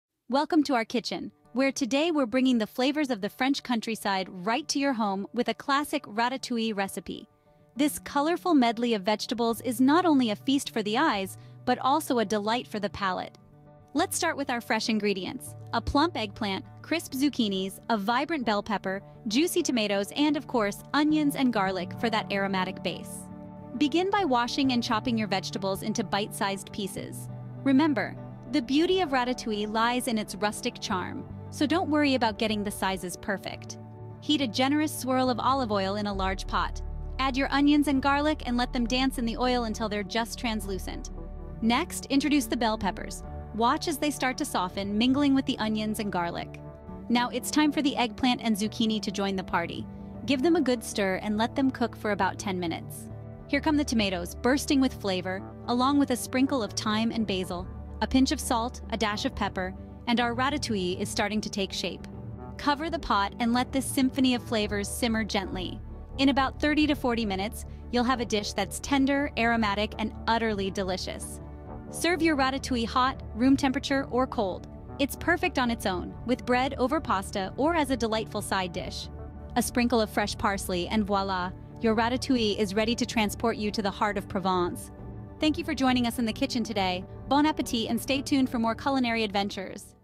Delight Your Senses with a Colorful Ratatouille (Mediterranean diet)
Welcome to our kitchen, where we'll show you how to bring the flavors of the French countryside to your home with a delicious ratatouille recipe. Get ready to feast your eyes on a colorful medley of fresh vegetables and indulge your palate with the perfect blend of flavors. Join us as we guide you step by step through this aromatic and mouthwatering dish.